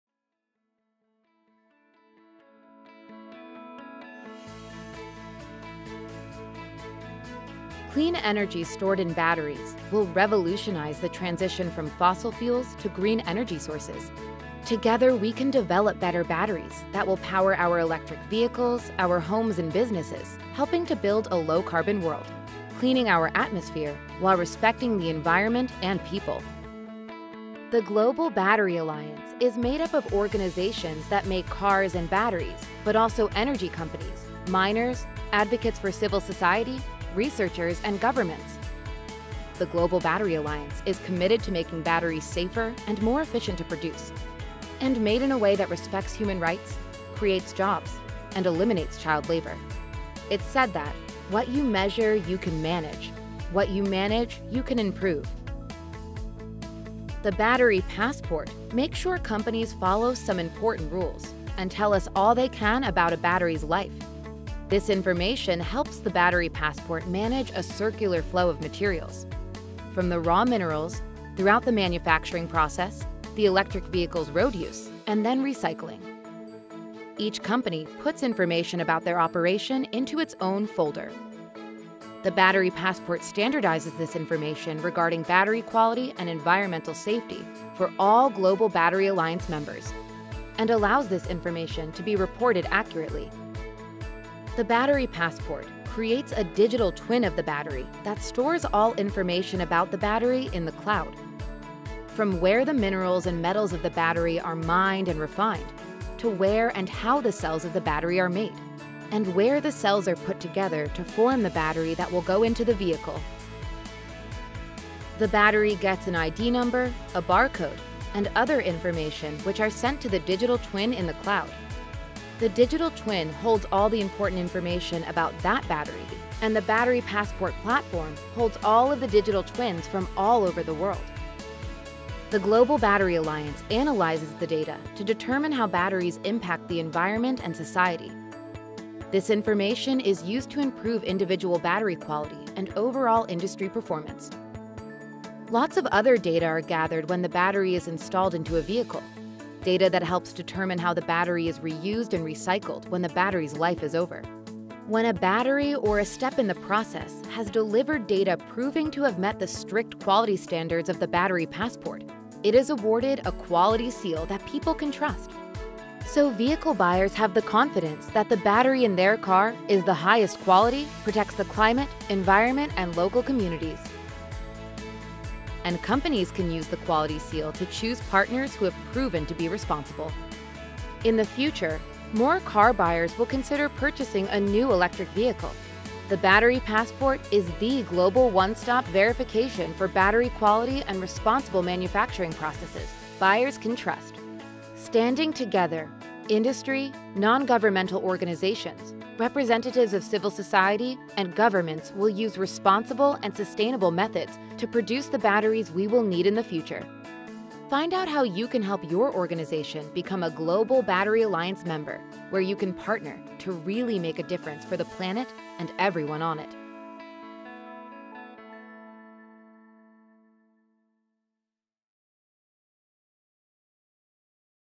Welcome to GBA's Battery Passport!
Batteries do much more than just power electric vehicles (EVs) – they also help determine their performance, service life, charging speed and costs. That is, however, only part of a battery’s much longer journey along the value chain, which starts with the extraction of raw materials and extends into the second phase of each battery’s life as stationary energy storage or recycling sources.

The production of a rechargeable battery – from mine to electric vehicle – poses significant social and environmental risks.  These range from mineral extraction (child labor, unsafe working conditions, indigenous rights), to production processes (C02 footprint, water use, biodiversity loss, pollution) and significantly impact the overall sustainability of the end product.

The Battery Passport is a digital representation of a battery that conveys information about all applicable ESG and lifecycle requirements based on a comprehensive definition of a sustainable battery. Each Battery Passport will be a digital twin of its physical battery enabled by the digital Battery Passport platform, which offers a global solution for securely sharing information and data. This platform aims to go beyond enabling the performance management of just one battery to that of all batteries across the full industry value chain.

The Battery Passport’s role in underpinning a responsible battery value chain has been endorsed during global policy discussions, including in the 2021 G7 Leaders’ Meeting, the draft EU Directive on Batteries and by the Canadian and US administrations.